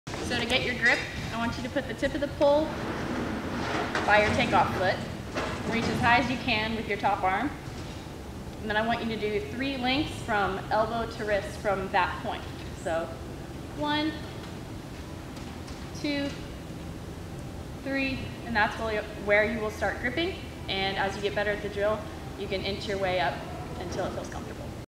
How to Pole Vault: How to Hold Pole by Bryan Compton
Bryan Compton is an American pole vault coach who has coached numerous Olympians and national champions, his most notable athlete is Sandi Morris the 2016 Olympic silver medalist in Rio. Bryan has created a workout series on Trackwired, check out his videos and workouts on trackwired.com or in the app store.